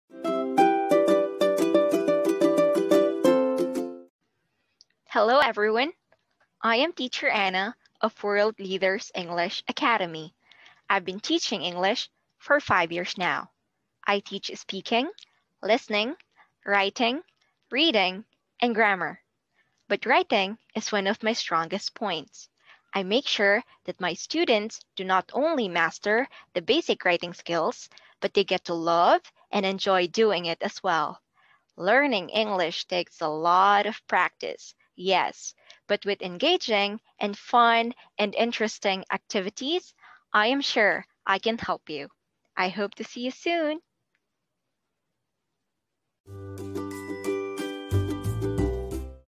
Meet Teacher Ana (ESL - Writing, Speaking, Grammar)
Meet Teacher Ana.

She teaches ESL  Writing, Speaking, Reading and Grammar. She leads the Creative Writing classes.